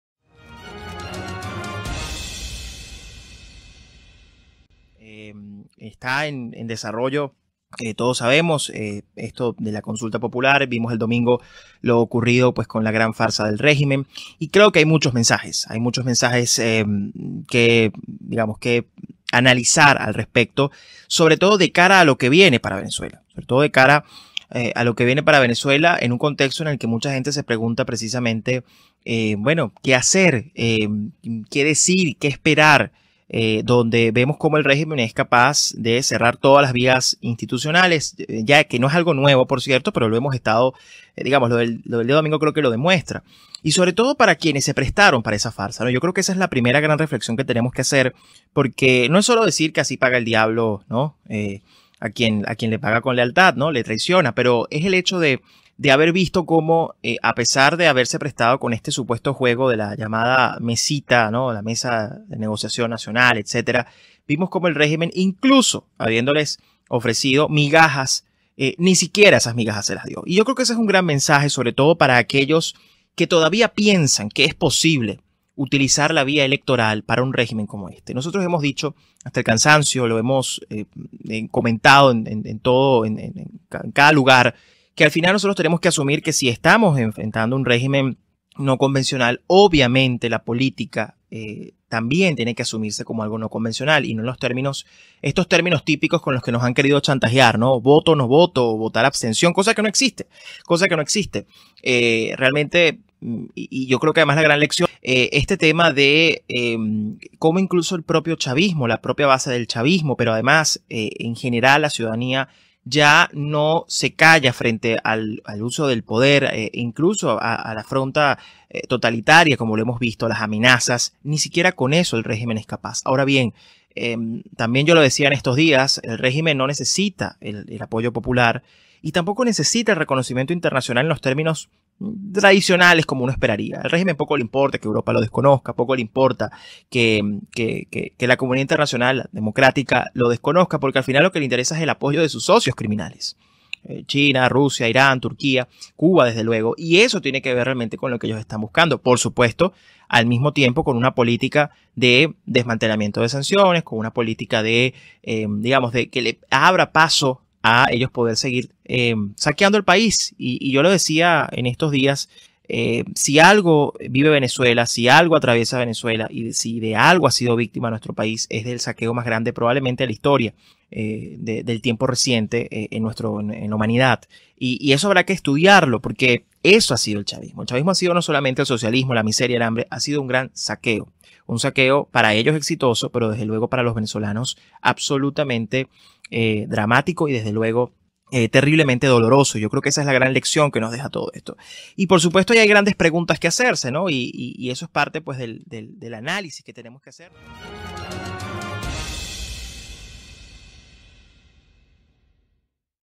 EDITORIAL PEDRO URRUCHURTU SOBRE LO QUE VIENE PARA VENEZUELA
Pedro Urruchurtu Editorial Diciembre 10